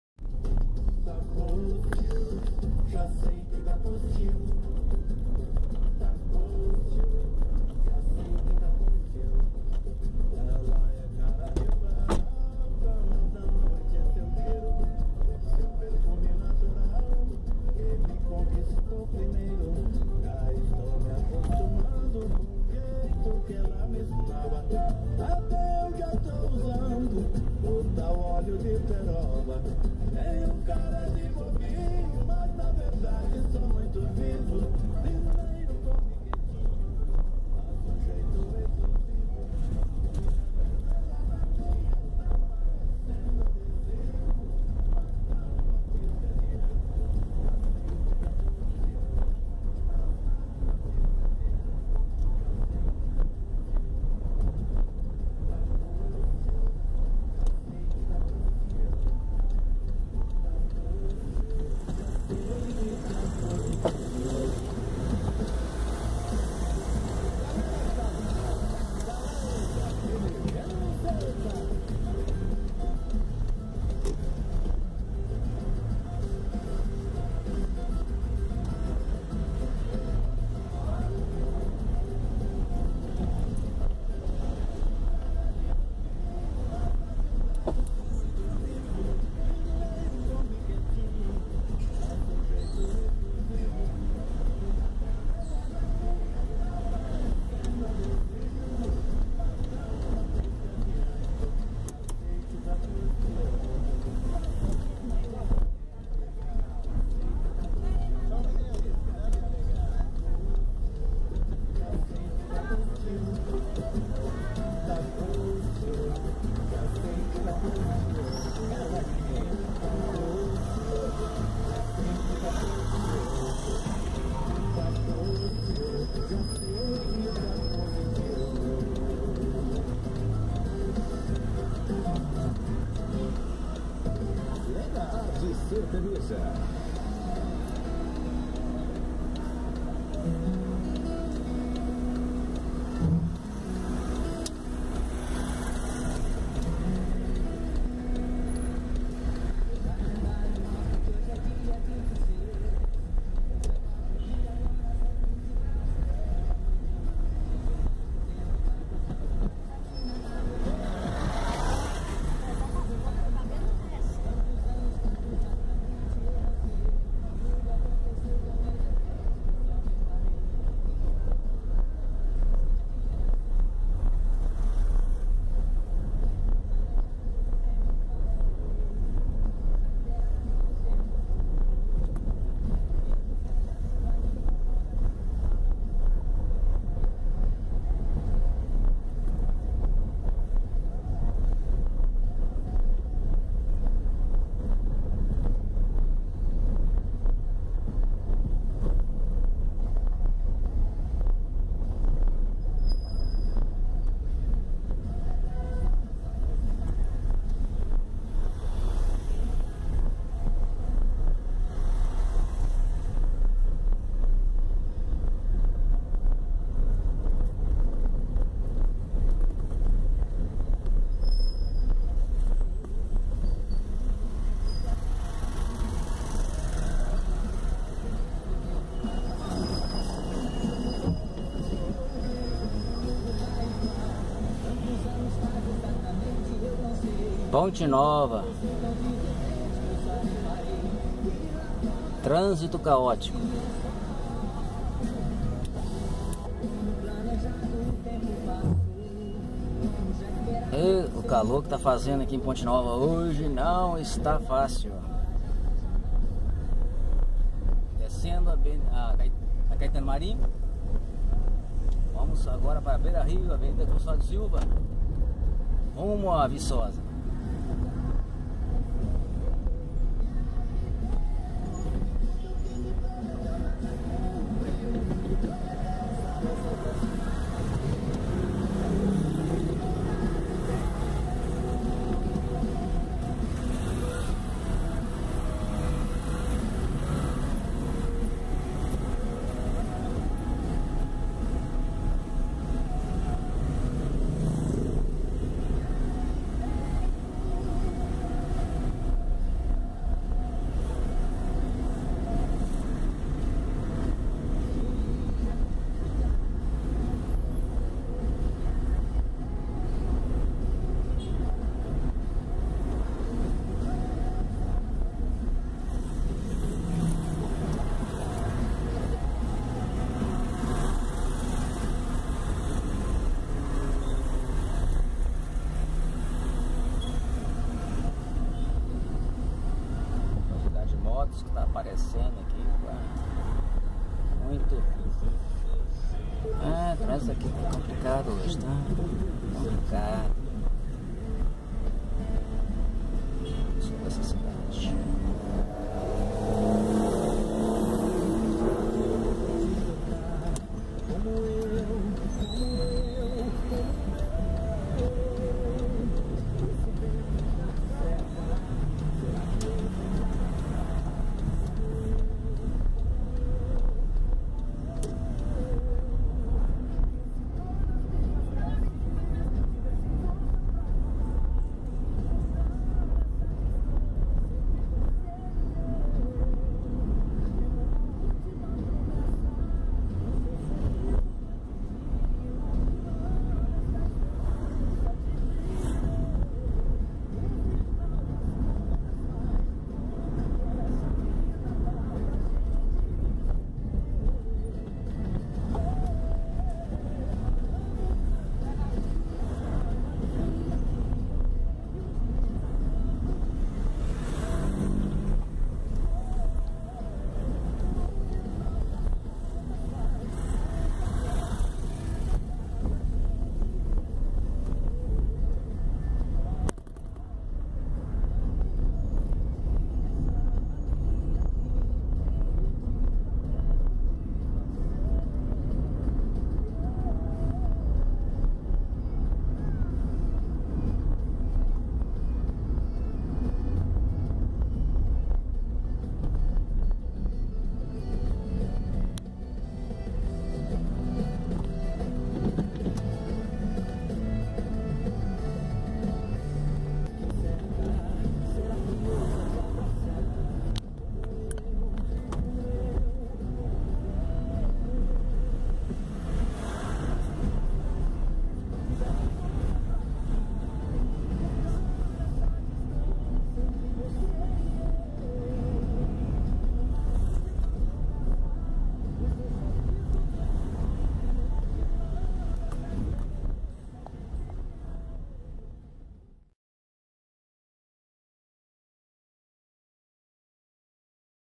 AQUI É MINAS GERAIS - PONTE NOVA - MG
Passeando pelas ruas de Ponte Nova. Visitando minha terra natal.